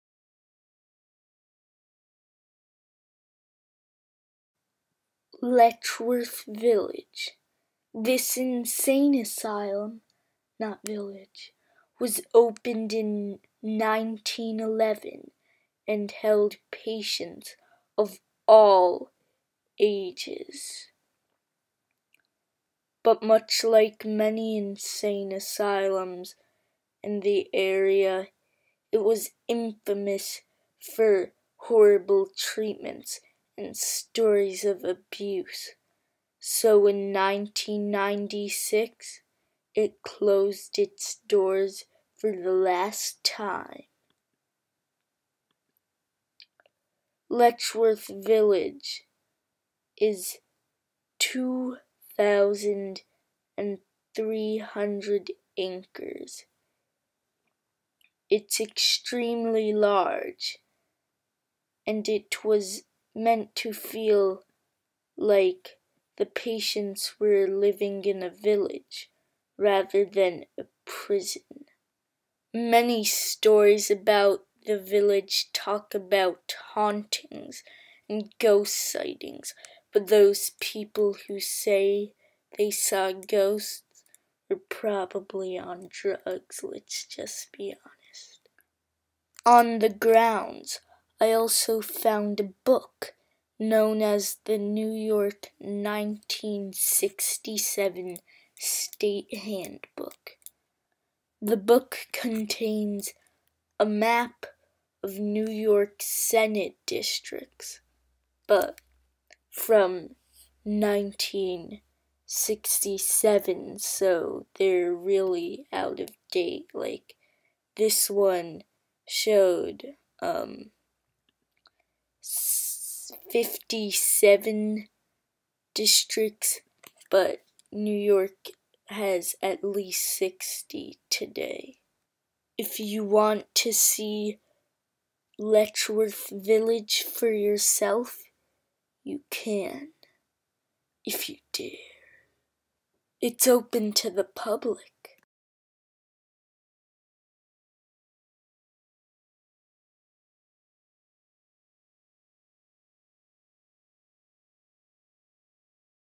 LetchWorth Village
Letchworth Village was an insane asylum in Thiells, NY. It was used to treat patients of all ages and it opened in 1911. Finally in 1996, after being open for 85 years Letchworth village closed its doors for the last time. You can still see Letchworth Village today, for it is now a park.